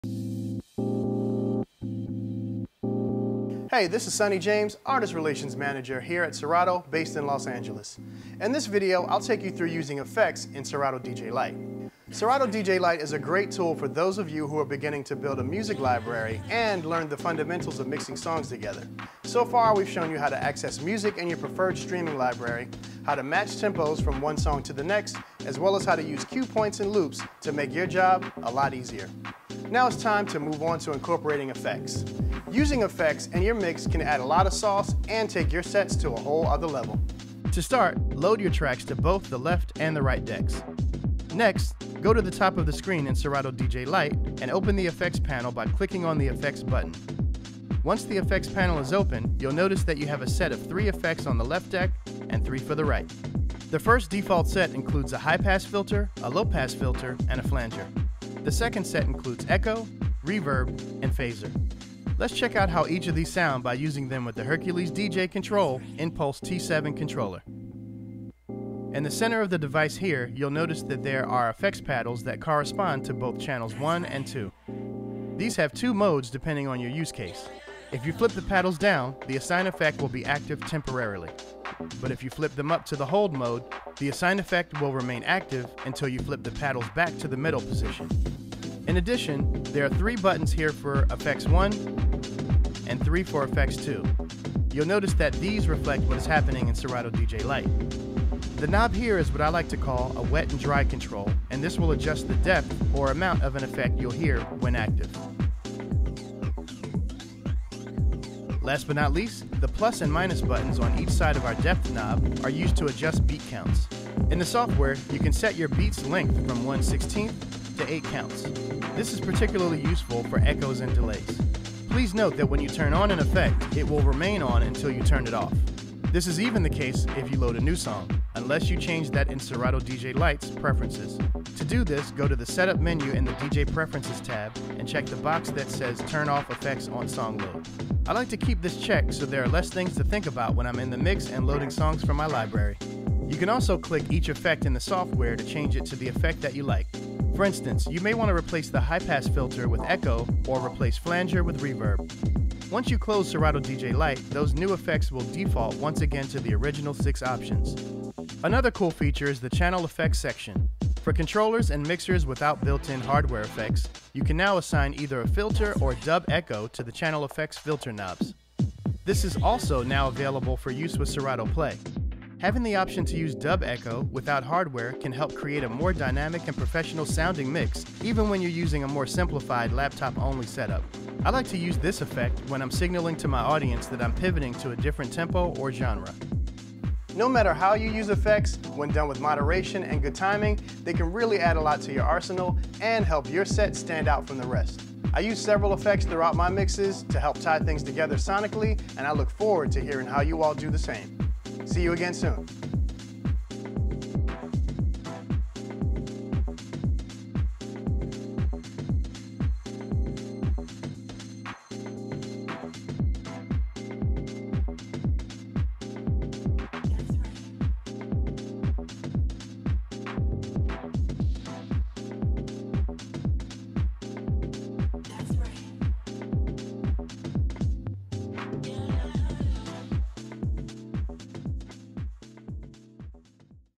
How to use FX in Serato DJ Lite
Learn how to use FX in Serato DJ Lite and make your DJ sets stand out.

0:00 Intro
0:38 Load a track to both decks
0:44 Open the FX panel
0:53 FX panel overview
1:09 Use FX with hardware
2:02 FX preferences
2:23 Changing FX
2:38 Channel FX
3:07 Outro

Try out even more FX in Serato DJ Pro - download

To see the full list of FX available with your subscription/license, including Suite/Expansions packs,